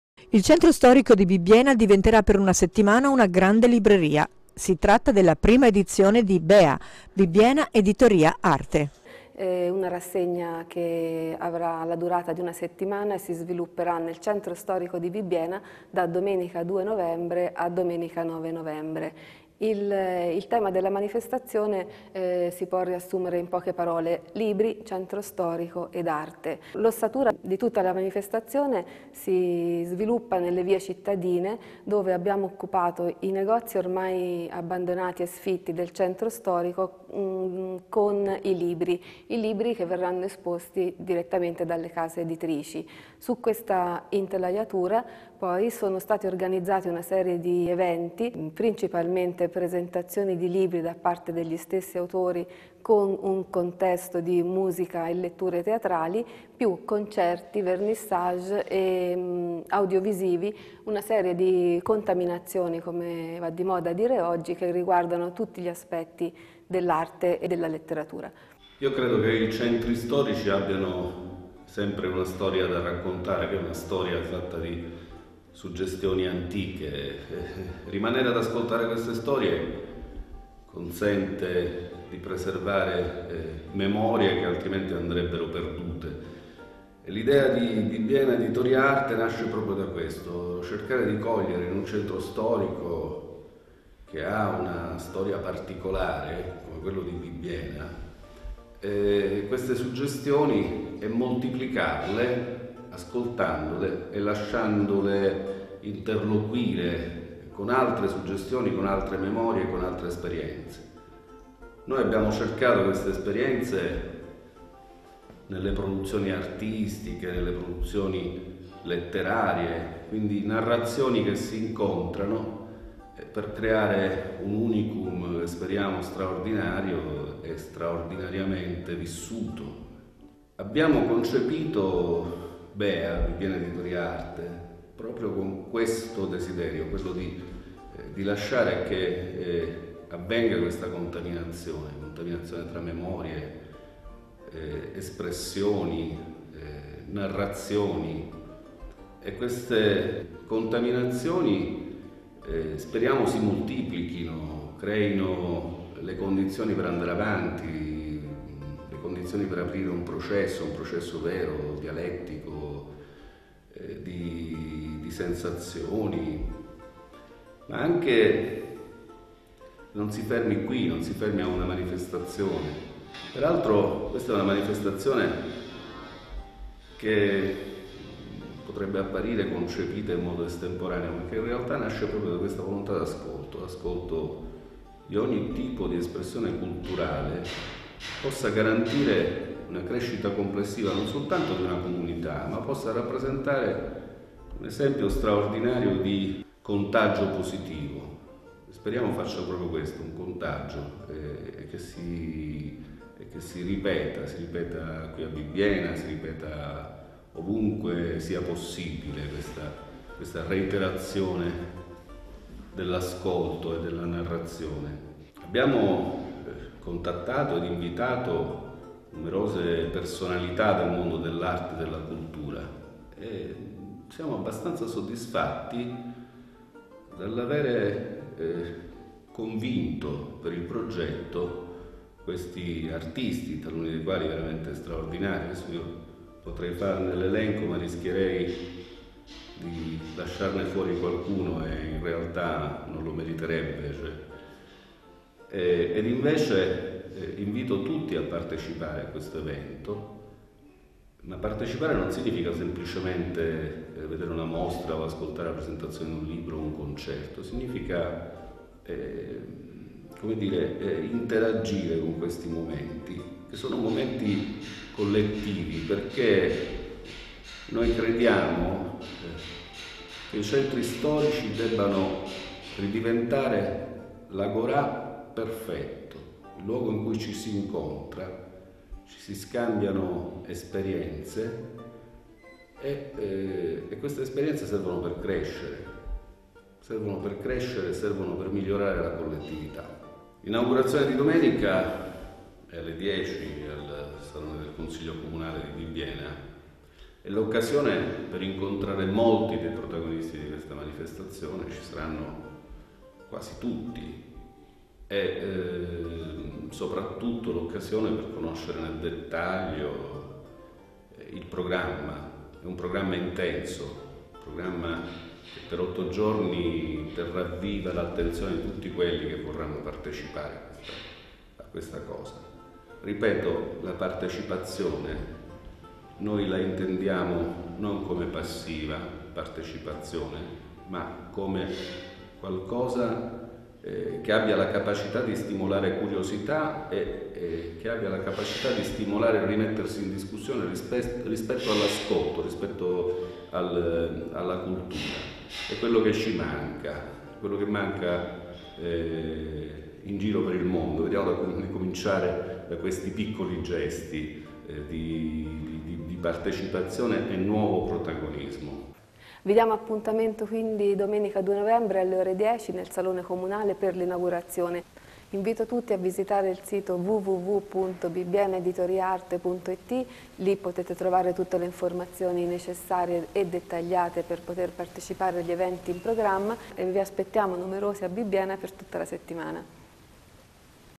Bibbiena: Bea Bibbiena editoria Arte dal 2 al 9 novembre
Un servizio in onda sul canale digitale terrestre di Teletruria 2 -Tvs  a cura dell' a.c. Centodue SubbianoTv realizzato per la promozione del territorio da Antonio Volpe